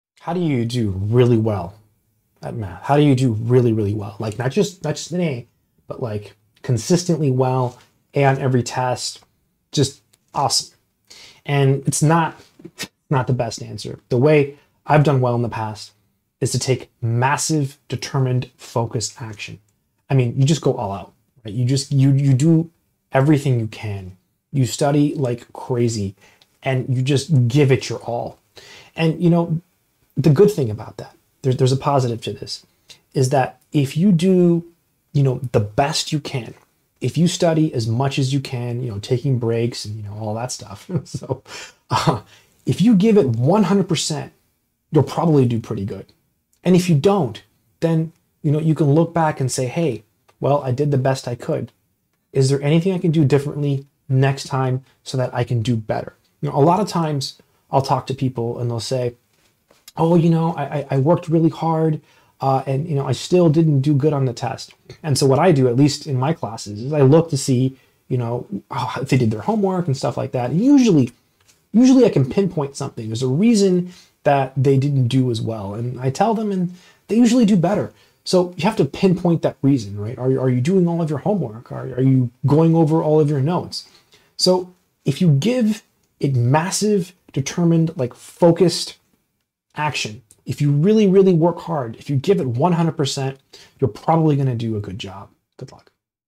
Doing Your Best In Math
Just a short video where I talk about doing well in math. I hope this helps someone.